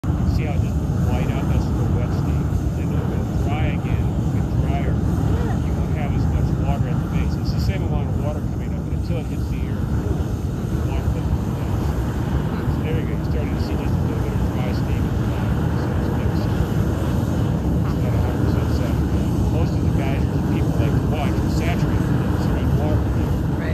Geyser in Yellowstone
This is a video I made in Yellowstone National Park in September 2024